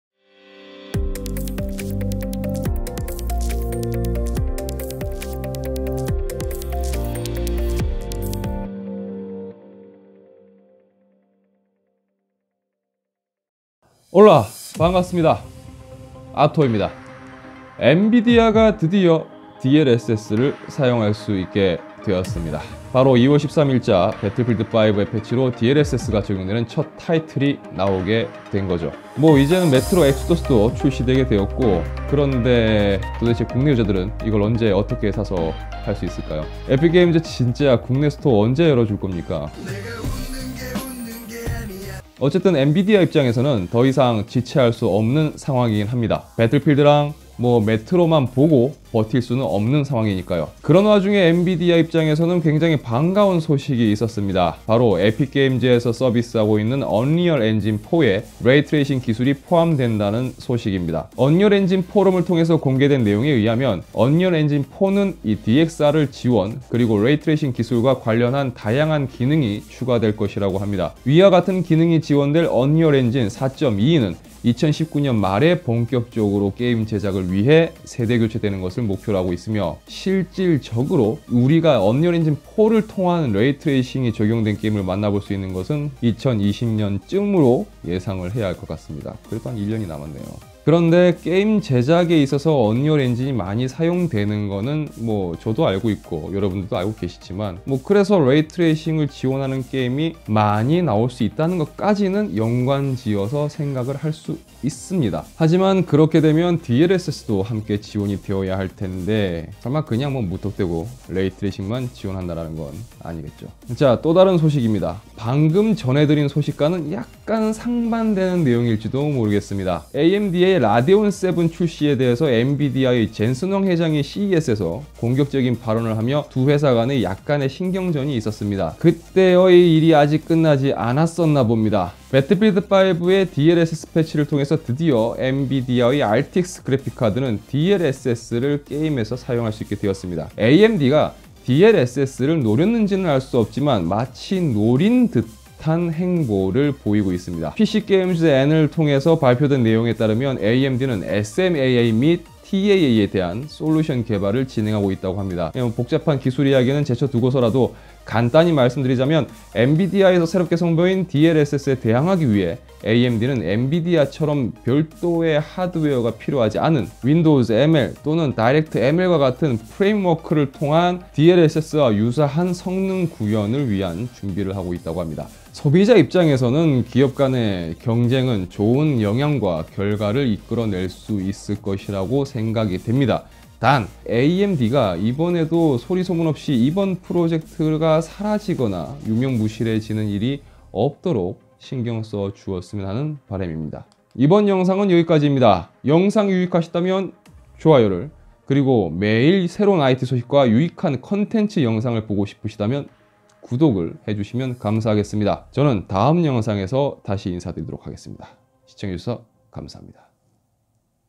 언리얼엔진도 레이트레이싱 본격 지원 그리고, DLSS 에 대항하는 AMD의 행보
언리얼 엔진에도 레이트레이싱이 추가되는 소식과 DLSS 에 대항하기위해 준비중인 AMD 의 행보에 대해서 알려드립니다.

참고자료 :
언리얼 포럼

- 편집 : 아토

- 장비
- Galaxy S8+